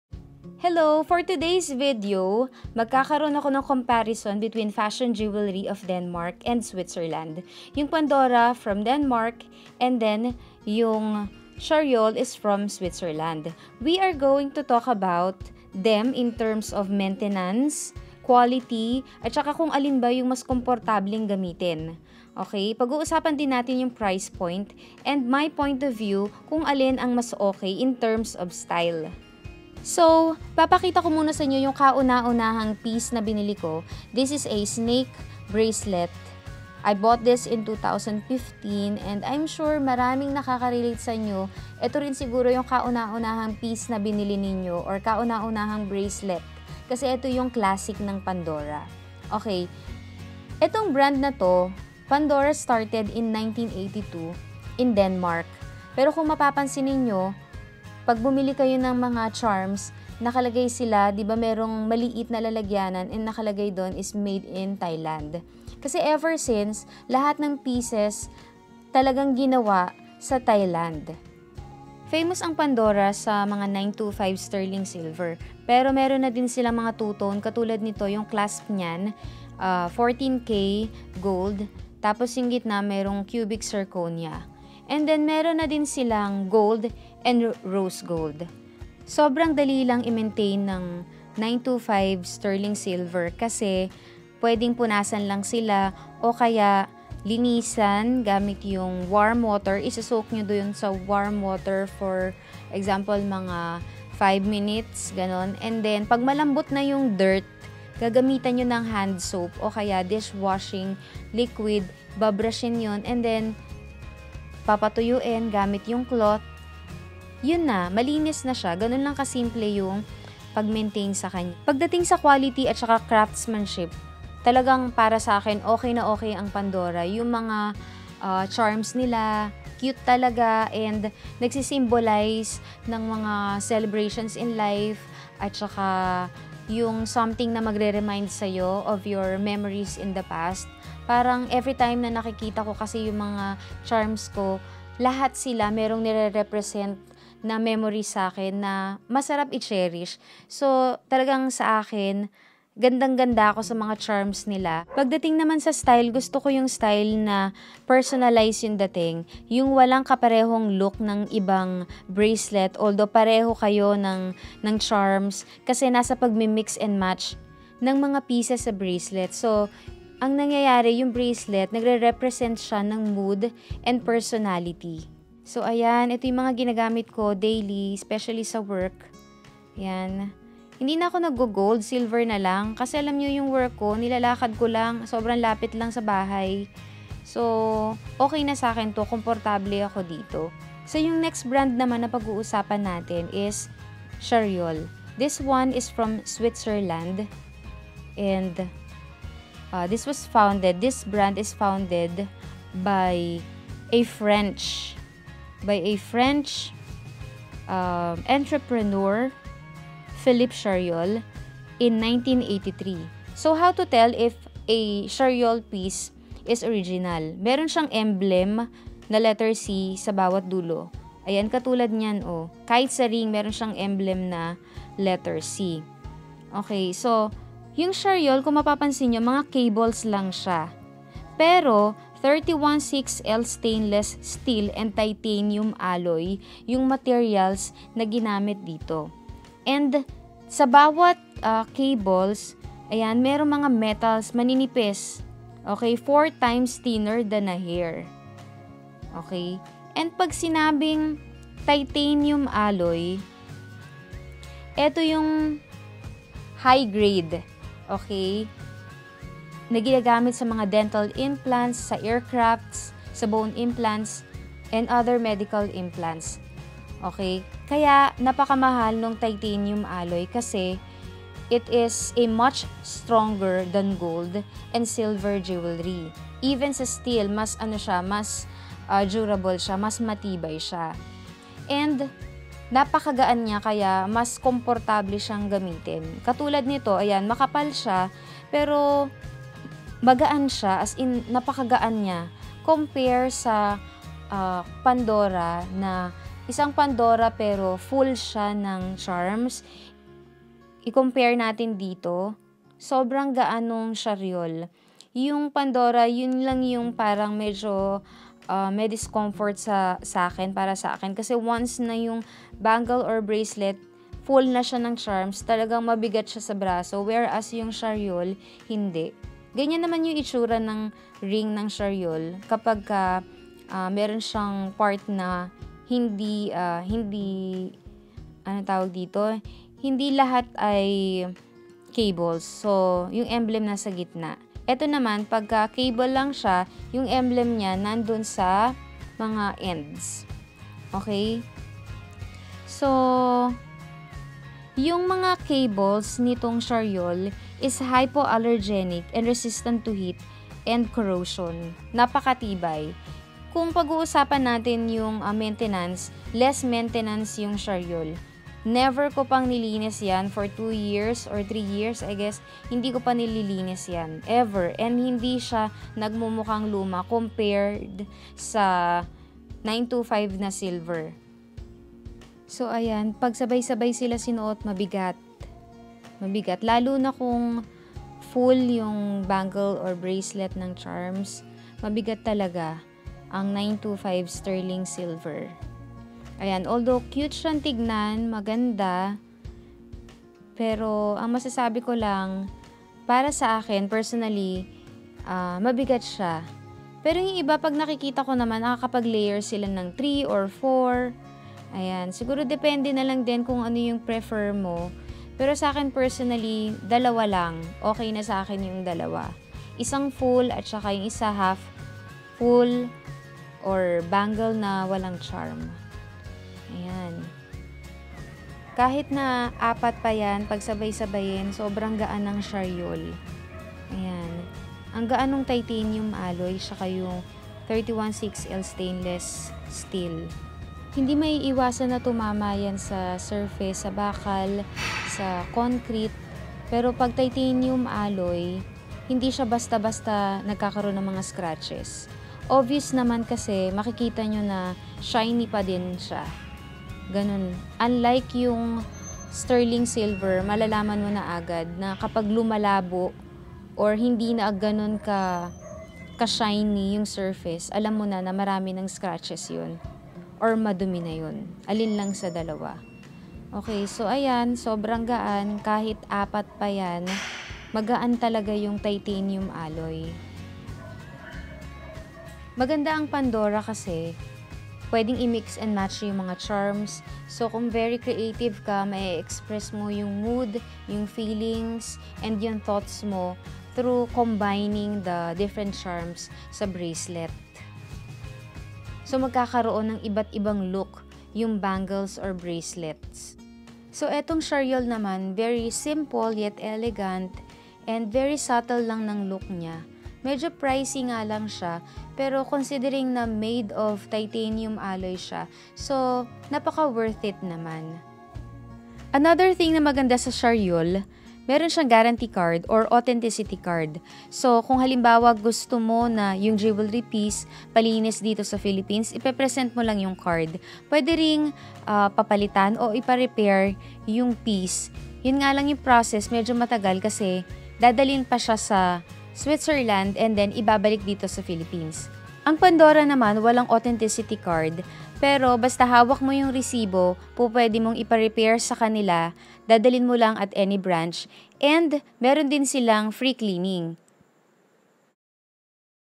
Comparison|Review | Sterling Silver 925 ale Jewelry vs.Titanium Alloy Jewelry | Pandora | Charriol
Fashion jewelry brands: Pandora and Charriol